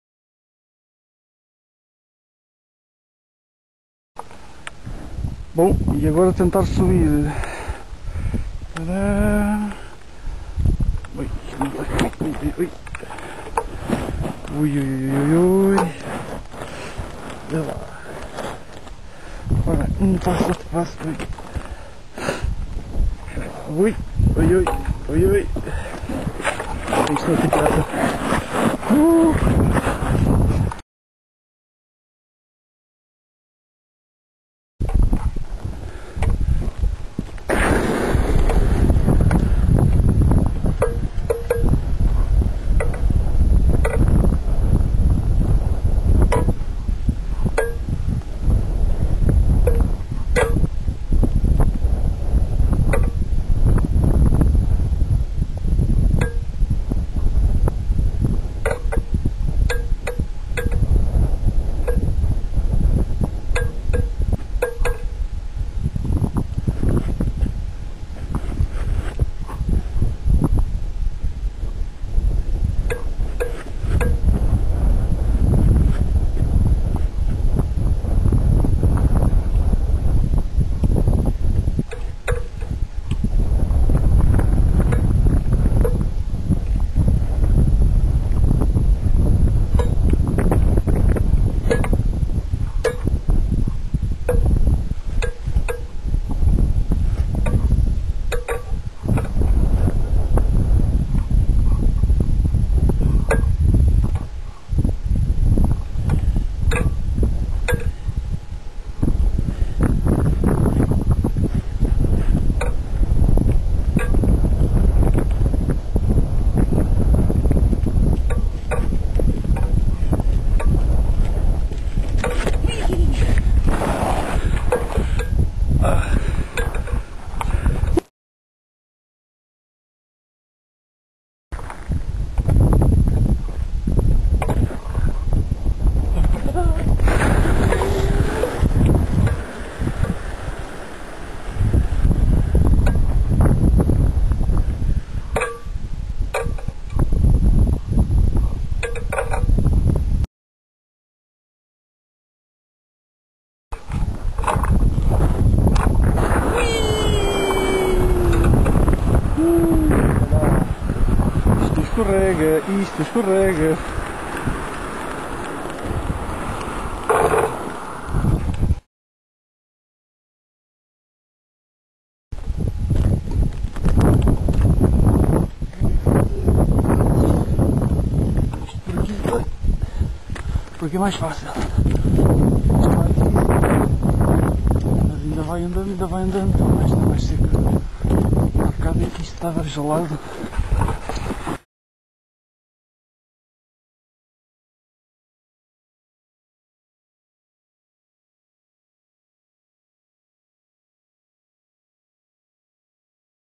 Icy Observatory
What do astronomers do when bad weather spoils their observations, and their trip home?
They have fun! Or if not, they should!!

Make sure you turn on English subtitles, by clicking the bottom right button, after the "volume" button! More fun that way!